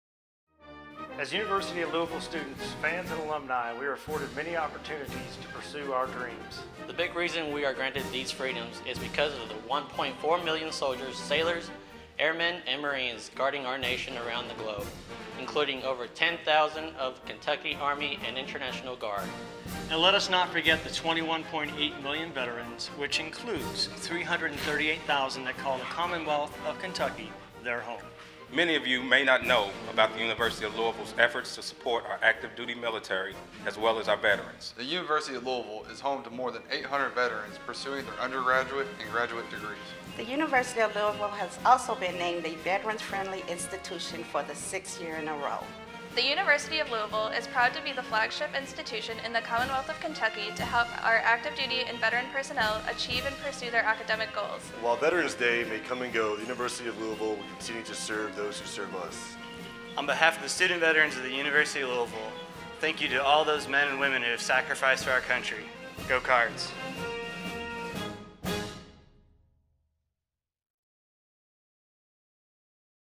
Louisville Student Veterans Tribute 2015
The University of Louisville is home to more than 800 student veterans pursuing their educational goals.  UofL's mission to smooth the transition from military life to student life is highlighted by a broad range of services.  The Louisville Cardinals salute these UofL student veterans for their service and are proud to be a part of the Louisville community with them.